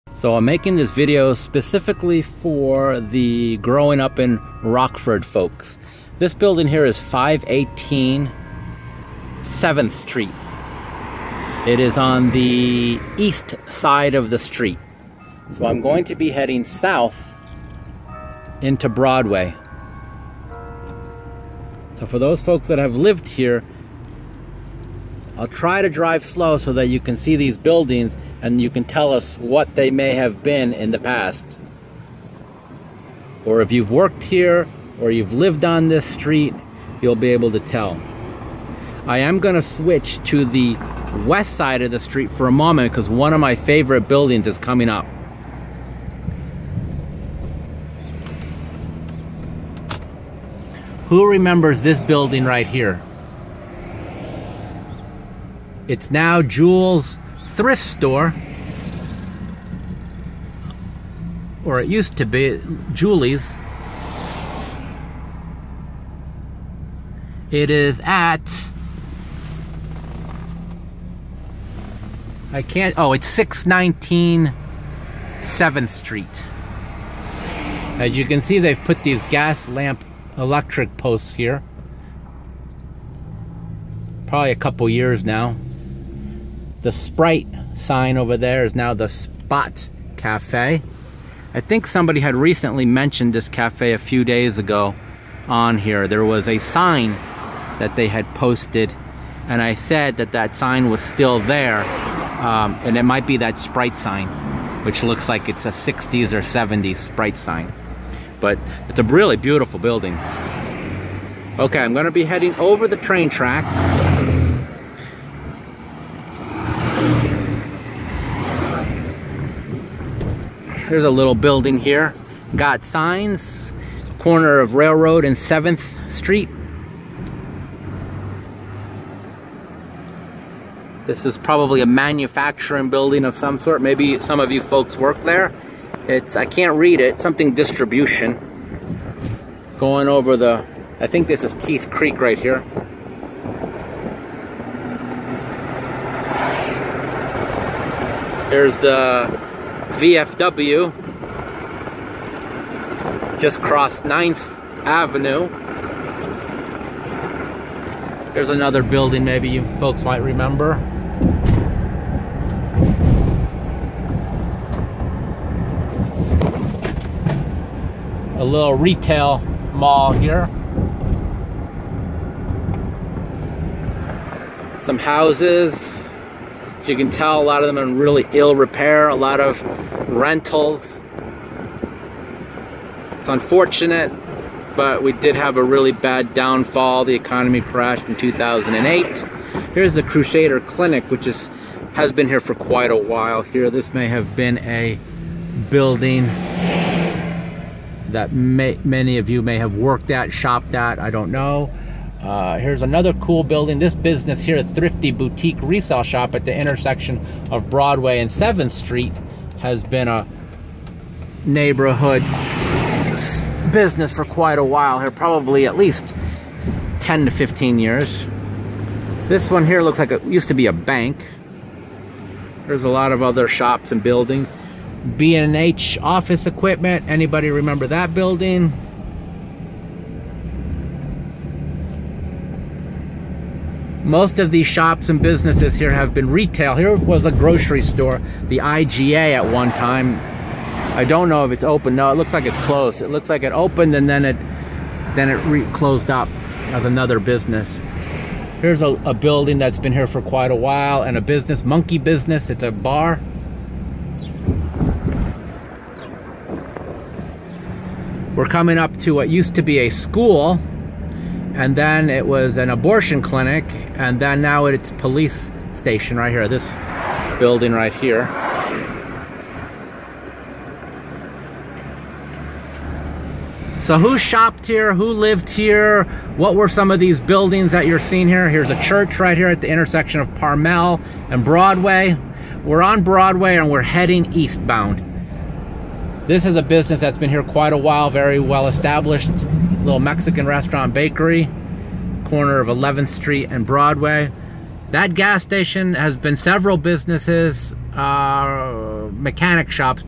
7th Street Rockford Illinois by
Drive Southbound of historic buildings. Who remembers this area?